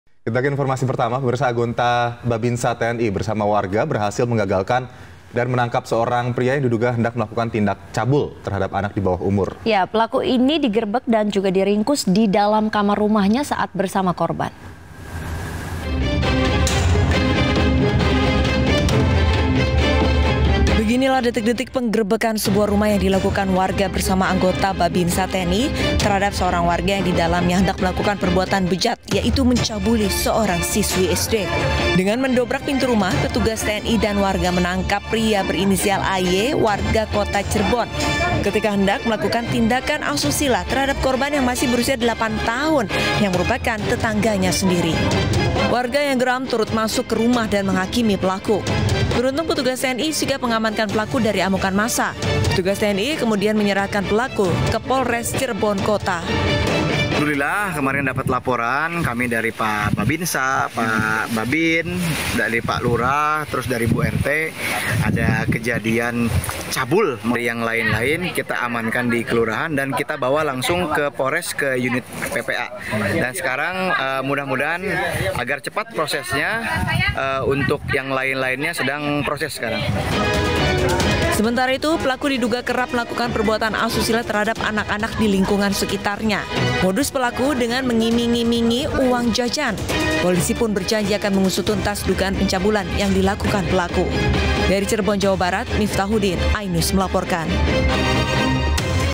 Detik-detik TNI di Cirebon Ringkus Pelaku Pencabulan Anak 8 Tahun
Anggota Babinsa TNI bersama warga berhasil menggagalkan dan menangkap seorang pria yang diduga hendak melakukan tindak cabul terhadap anak dibawah umur berusia delapan tahun.

apatkan sajian berita-berita liputan langsung dari lapangan dan peristiwa terkini secara cepat dan akurat dari seluruh Indonesia. Baca berita-berita terpopuler di sini: